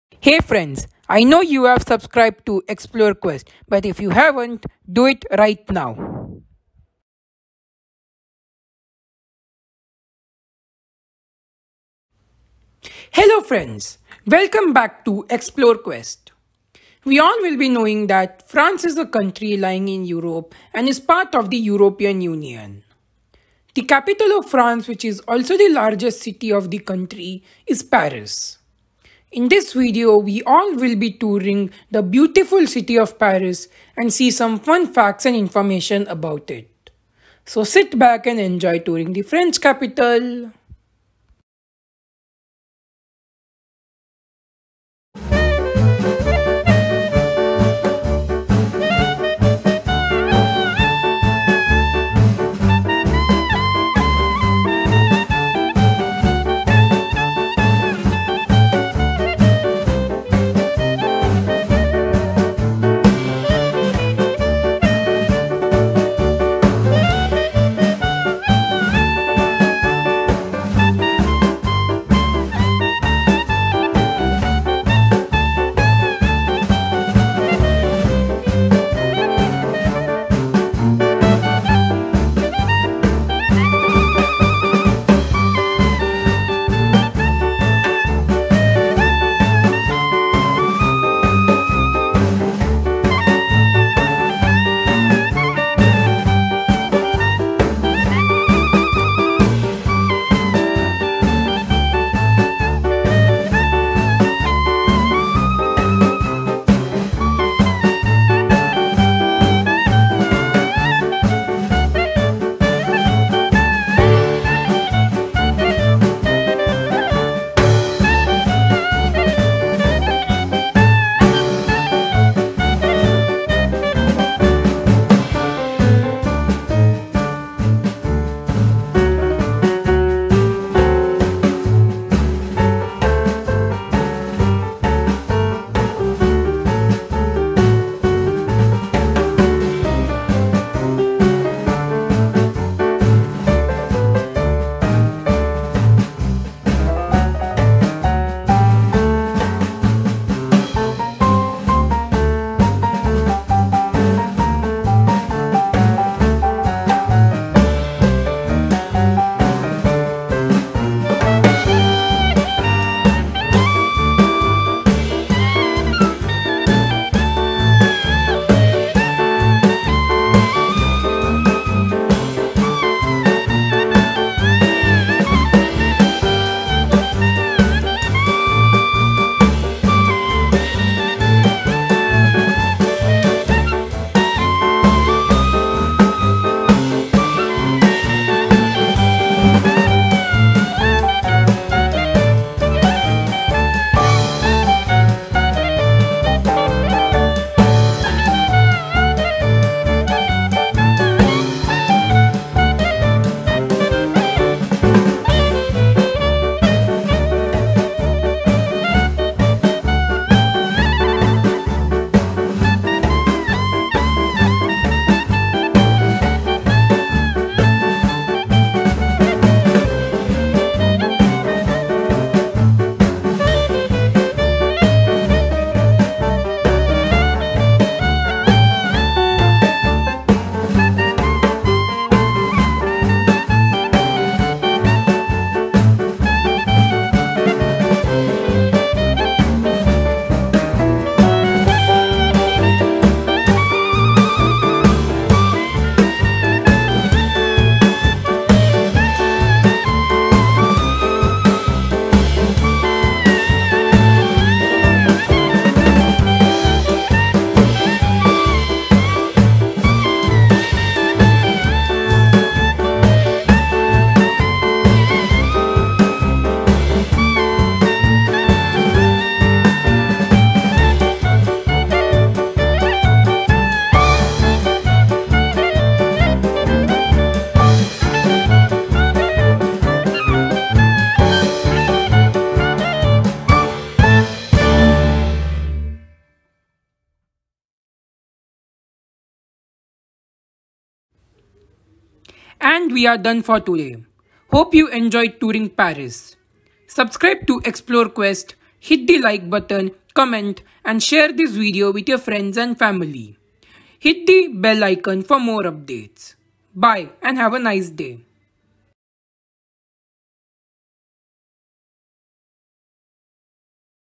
Within the French capital – Paris tour
Paris is the capital and most populous city of France. It is one of the most visited city in the world. In this video, we will be touring the French capital and see some fun facts and information.
Do Enjoy.

SHARE MY VIDEO WITH YOUR FRIENDS AND FAMILY